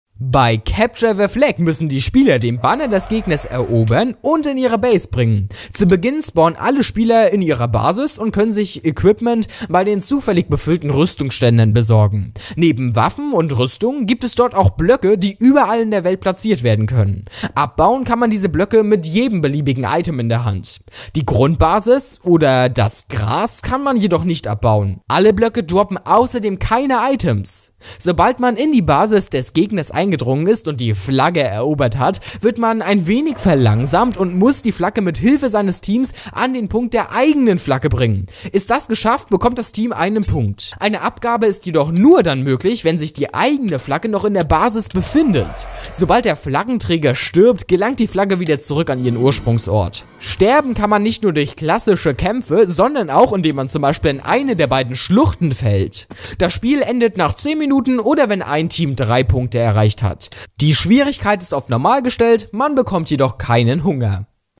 Regeln von Capture The Flag | 4fup Minigame-Map
Bei Capture The Flag müssen die Spieler den Banner des Gegners erobern und in ihre Basis bringen. Zu Beginn spawnen alle Spieler in ihrer Basis und können sich Equipment bei den zufällig befüllten Rüstungsständern besorgen.
Neben Waffen und Rüstungen gibt es dort auch Blöcke, die überall auf der Welt platziert werden können. Abbauen kann man diese Blöcke mit jedem beliebigen Item in der Hand. Die Grundbasis oder das Gras kann man jedoch nicht abbauen. Alle Blöcke droppen außerdem keine Items.
Sobald man in die Basis des Gegners eingedrungen ist und die Flagge erobert hat, wird man ein wenig verlangsamt und muss die Flagge mit der Hilfe seines Teams an den Punkt der eigenen Flagge bringen. Ist das geschafft, bekommt das Team einen Punkt. Eine Abgabe ist jedoch nur dann möglich, wenn sich die eigene Flagge noch in der Basis befindet. Sobald der Flaggenträger stirbt, gelangt die Flagge wieder zurück an ihren Ursprungsort. Sterben kann man nicht nur durch klassische Kämpfe, sondern auch, indem man in eine der beiden Schluchten. Das Spiel endet nach 10 Minuten oder, wenn ein Team 3 Punkte erreicht hat.
Die Schwierigkeit ist auf normal gestellt, man bekommt jedoch keinen Hunger.